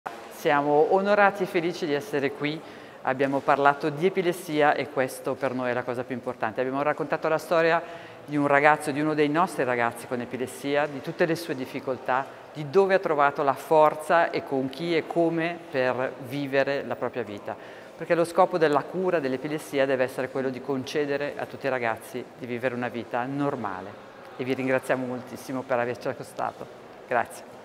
Intervista a Laura Tassi - LICE - Giffoni Next Generation 2023
L'intervista a Laura Tassi, Presidente di LICE, in occasione dell'anteprima del corto "Fuori dall'acqua"